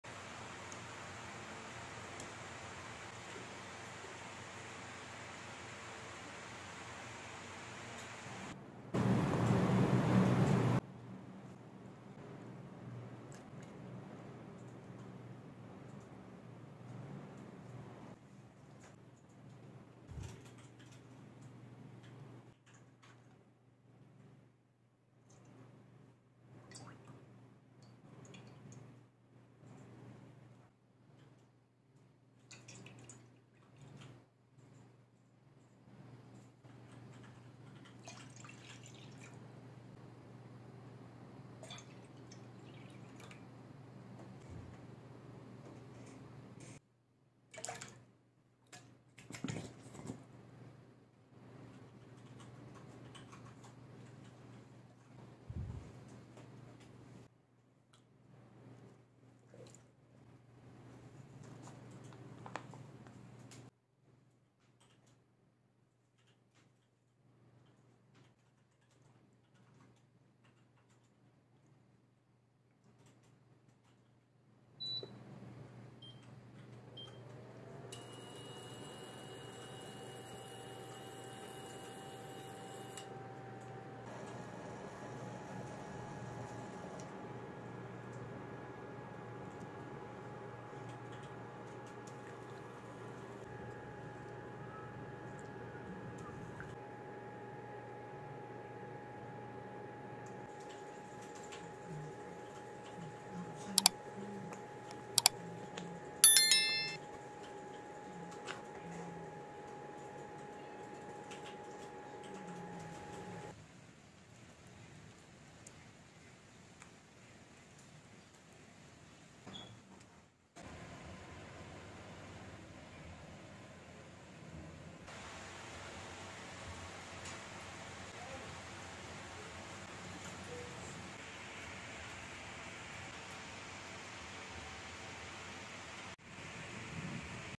Maja Blanca
Easy Maja Blanca Recipe

Ingredients:

400ml Jolly Coconut Milk
370ml Evaporated Milk
2 cups of water
1 can Jolly Cream Corn
1 can Jolly Whole Corn Kernels
1/4 cup Sweet Best Erythritol Natural Sweetener
Grated Cheese for toppings

For Slurry:
1 cup of water + 200g cornstarch